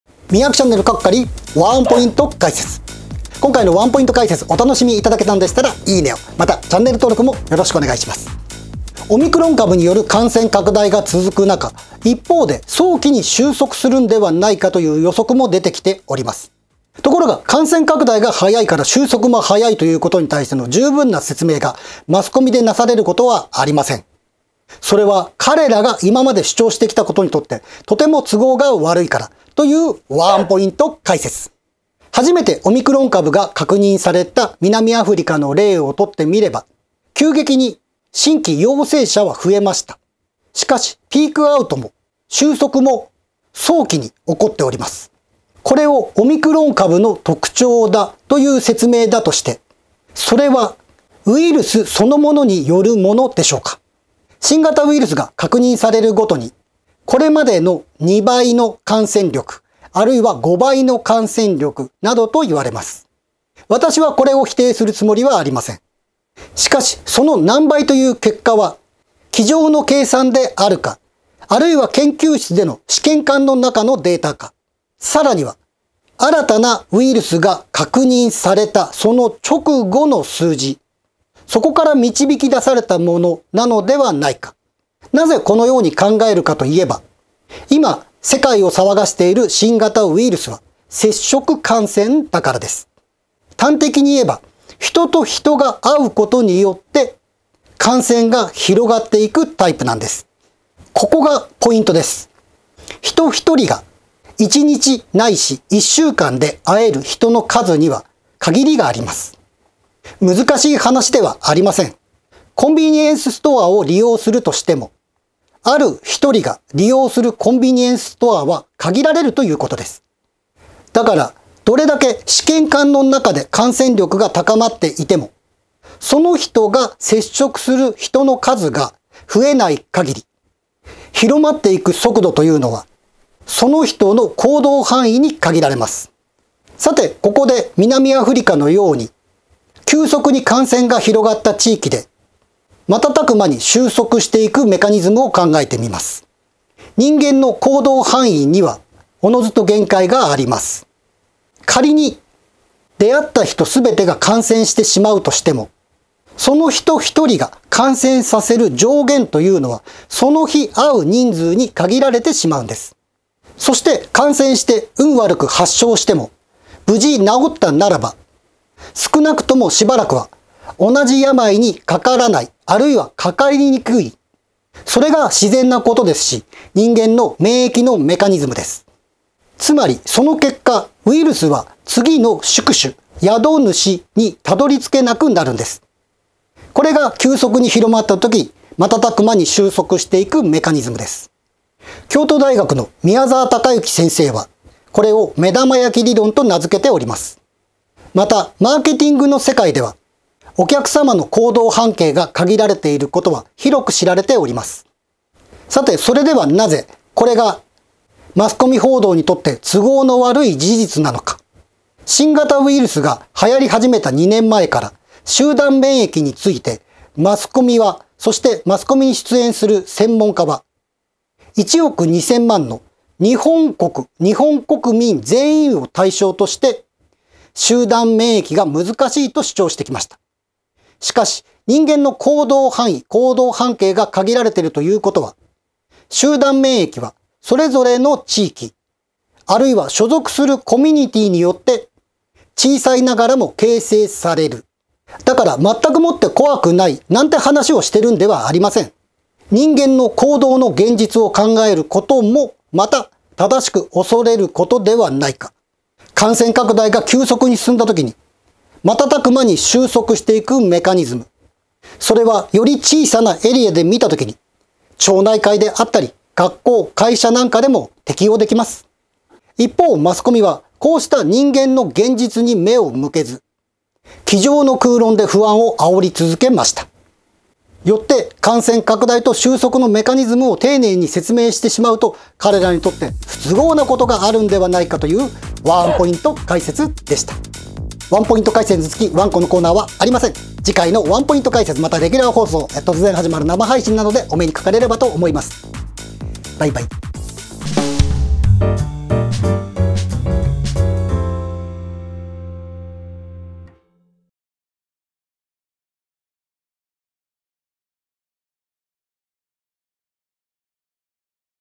南アのピークアウトと「集団免疫」の現実。マスコミに不都合な真実｜【３分解説】ワン☆ポイント解説#025
南アフリカもニューヨークもピークアウト。第６波の現実は「行動半径」に確認できる。「集団免疫」の現実｜【３分解説】ワン☆ポイント解説#025

■YouTubeでの限定動画についてはこちらから。
↓よろしければどうぞ。